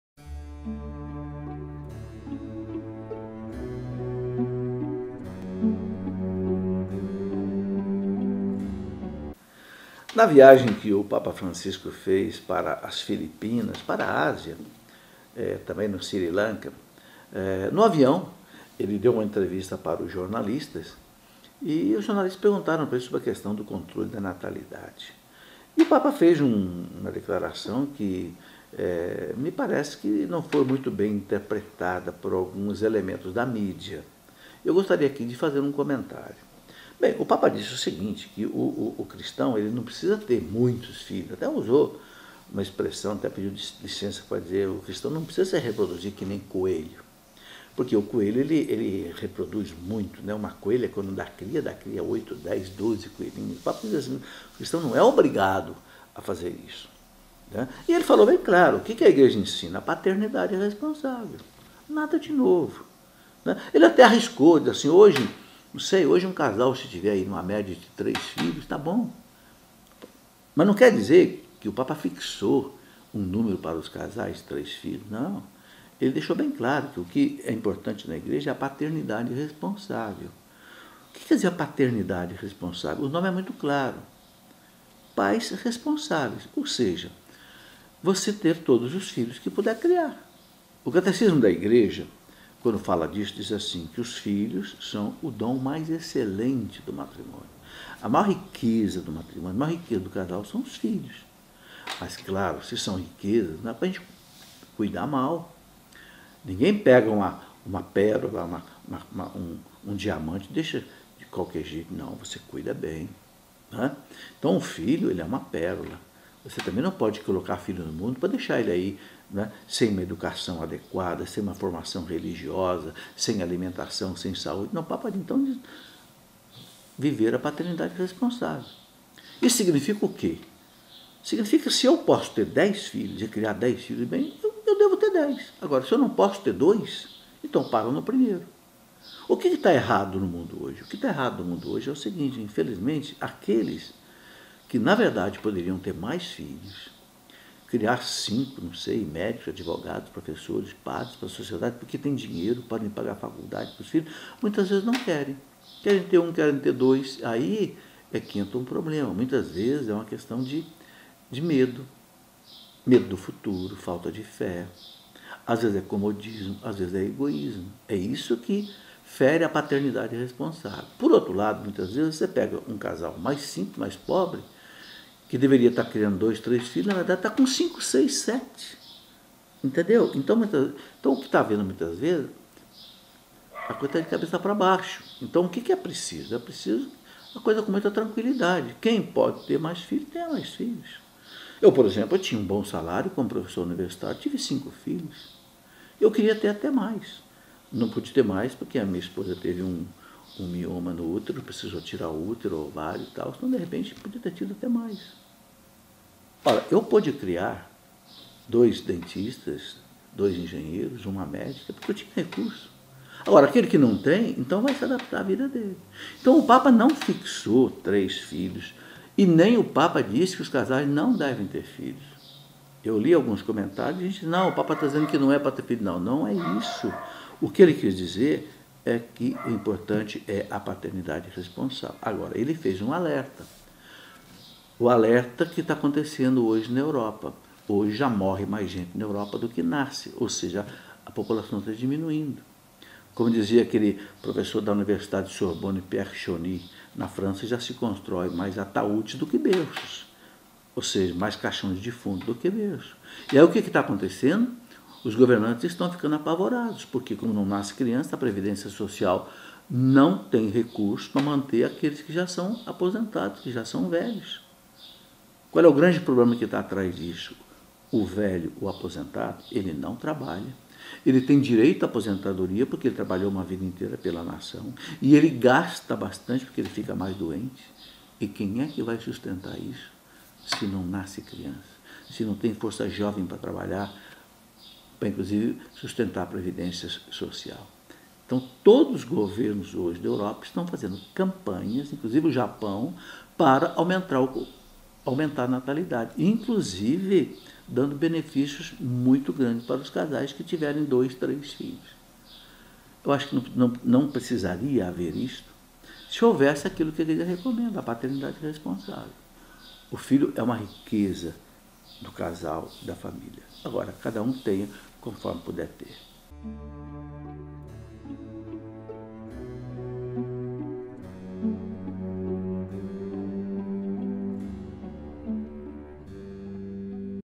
O que o Papa Francisco fala sobre o controle da Natalidade
Na viagem que o Papa Francisco fez para as Filipinas, ele deu uma entrevista sobre o controle da natalidade e uma de suas declarações não foi bem interpretada pela mídia.

Neste vídeo, o Prof. Felipe Aquino comenta sobre a entrevista e fala sobre a Paternidade Responsável.

-Instagram Ed. Cléofas:@editoracleofas_loja
-Instagram Prof. Felipe Aquino: @prof.felipe_aquino